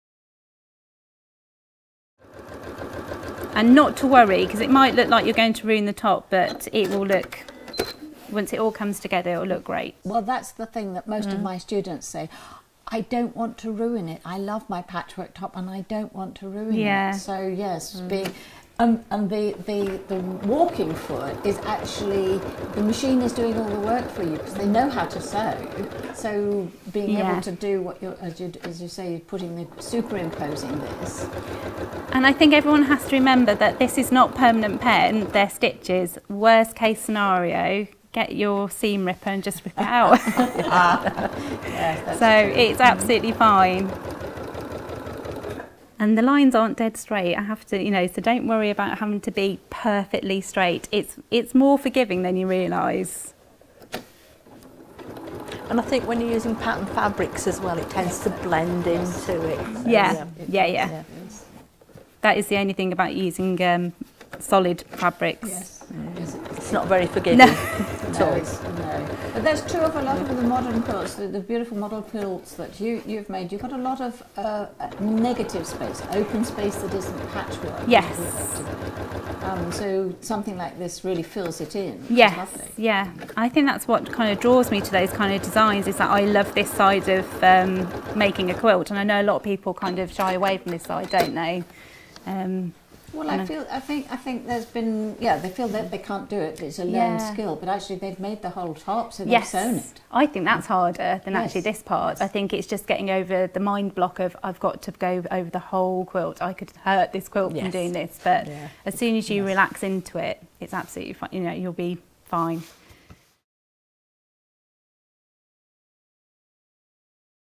You Won't Ruin Your Patchwork Top When You Quilt with Claire Campion
You Won't Ruin Your Patchwork Top When You Quilt with Claire Campion - she talks to Valerie and Jayne about overcoming the mind block of quilting your top.
She describes how to follow straight lines using your Walking Foot. The resulting quilt patterns are more forgiving than you realise. Giving an even quilting pattern that easily fills negative space.
When the Walking Foot does all the hard work, you can relax into your quilting.